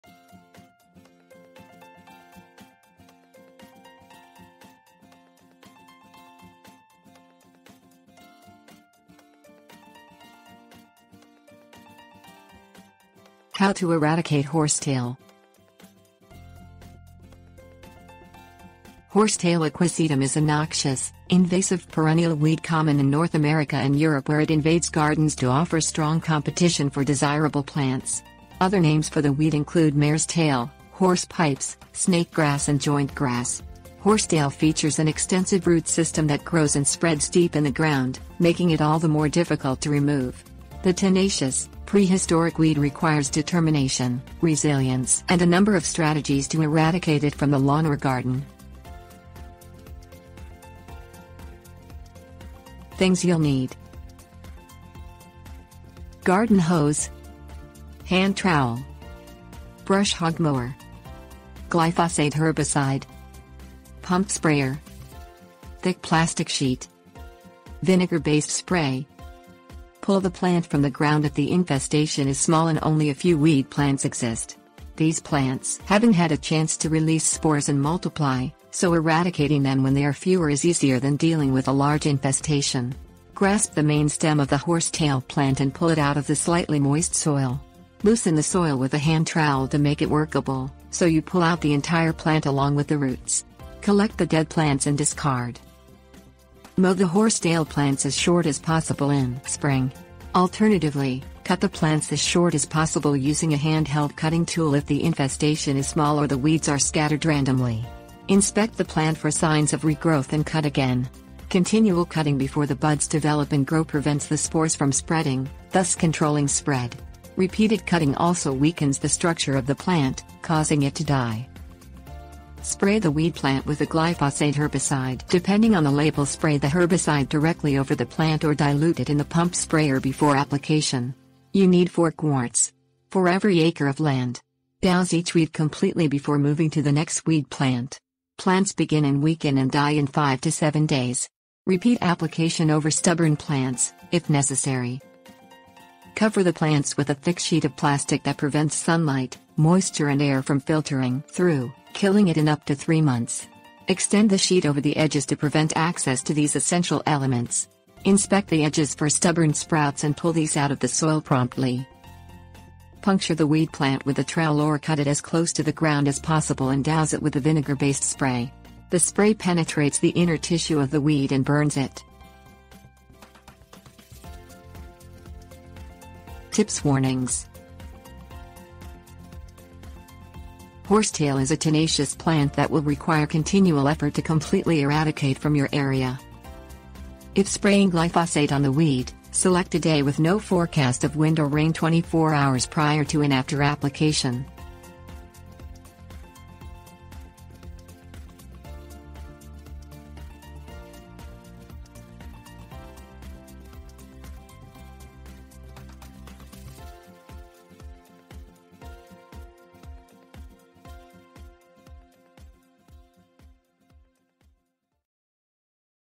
How to Eradicate Horsetail
How to Eradicate Horsetail. Horsetail (Equisetum) is a noxious, invasive perennial weed common in North America and Europe where it invades gardens to offer strong competition for desirable plants. Other names for the weed include mare's tail, horse pipes, snake grass and joint grass. Horsetail features an extensive root system that grows and...

Table of contents How to Eradicate Horsetail
Things You'll Need 00:51
Tips & Warnings 02:55